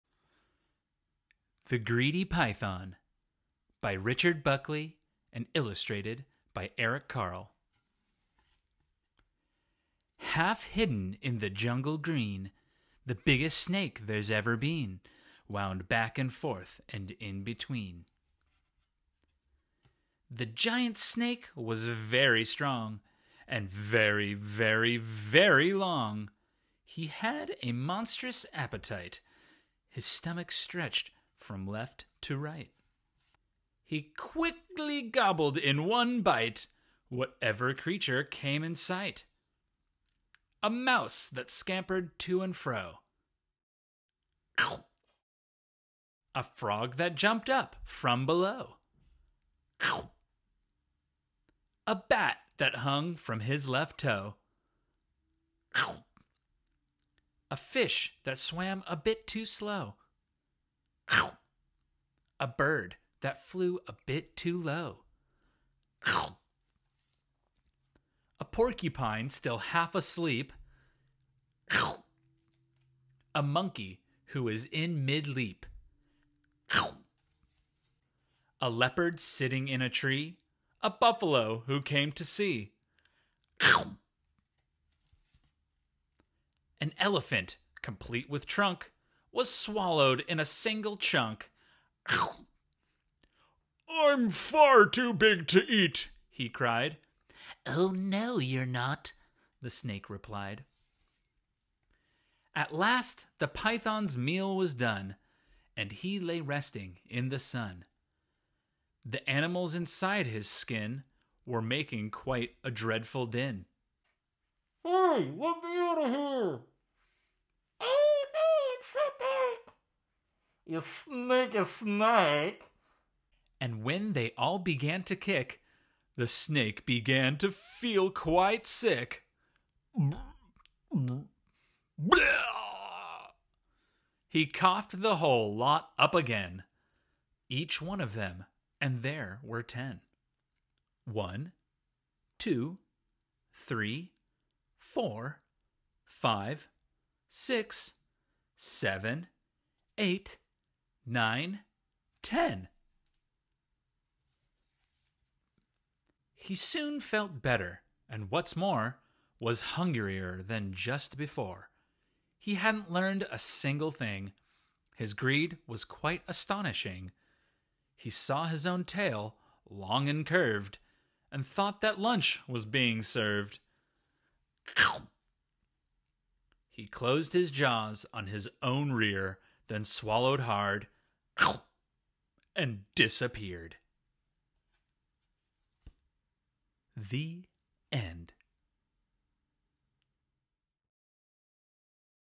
The Greedy Python | Animated Book | Eric Carle
The ravenous greedy python eats every creature he sees. From a tiny mouse to an enormous elephant, nothing is ever too big for this snake!